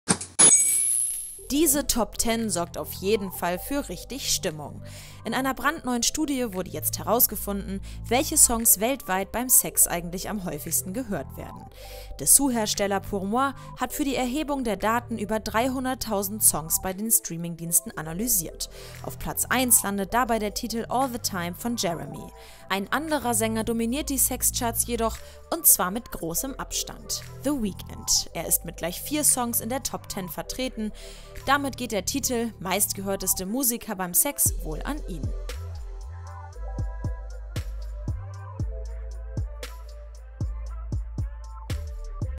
Enthüllt: Seine Songs werden beim Sex am häufigsten gehört | DeinUPDATE
Diese Top10 sorgt auf jeden Fall für die richtig Stimmung! In einer brandneuen Studie wurde jetzt herausgefunden, welche Songs weltweit beim Sex eigentlich am häufigsten gehört werden…